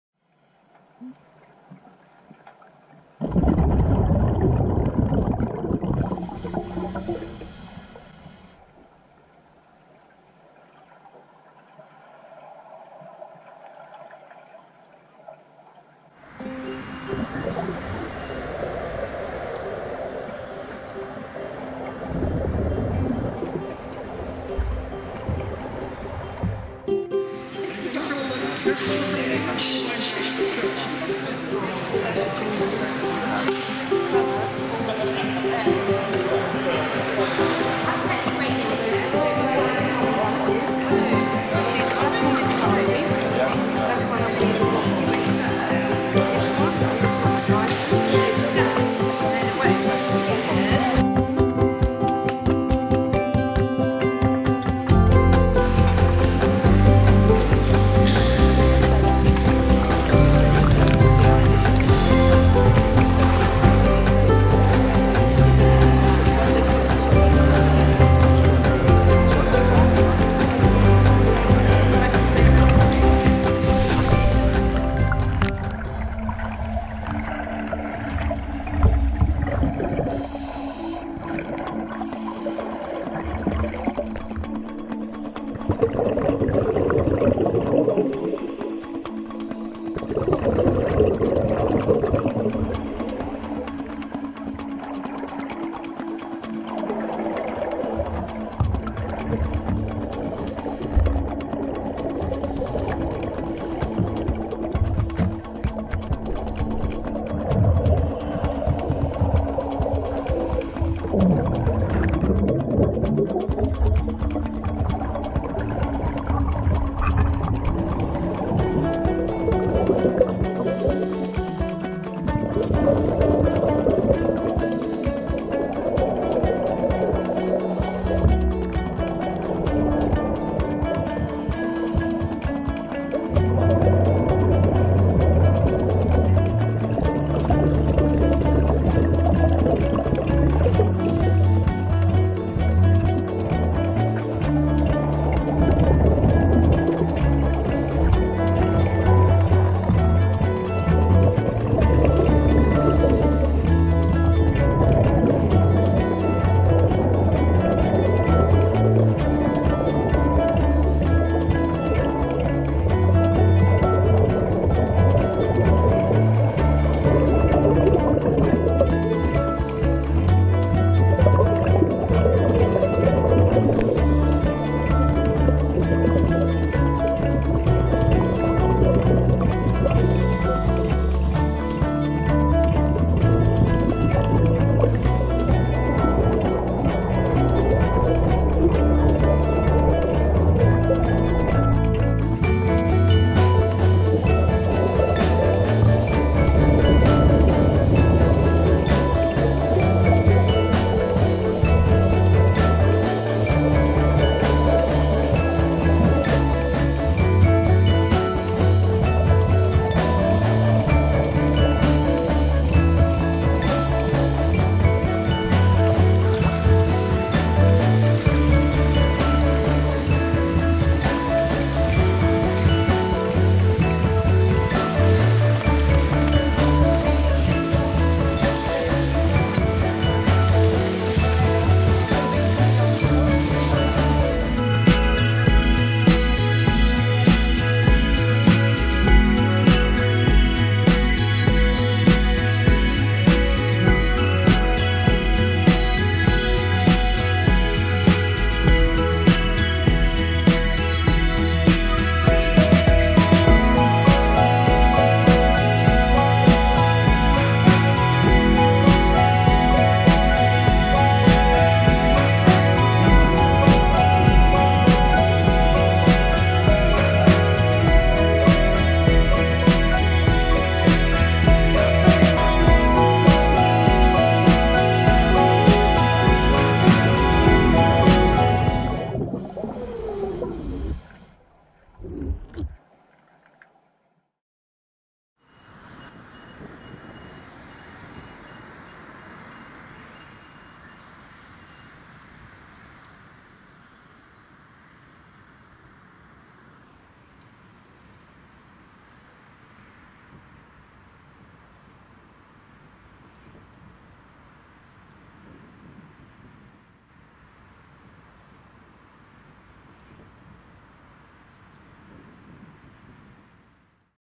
Try Dive November 2018
Club Try Dive at the Gosport Leisure Centre on 17th November 2018.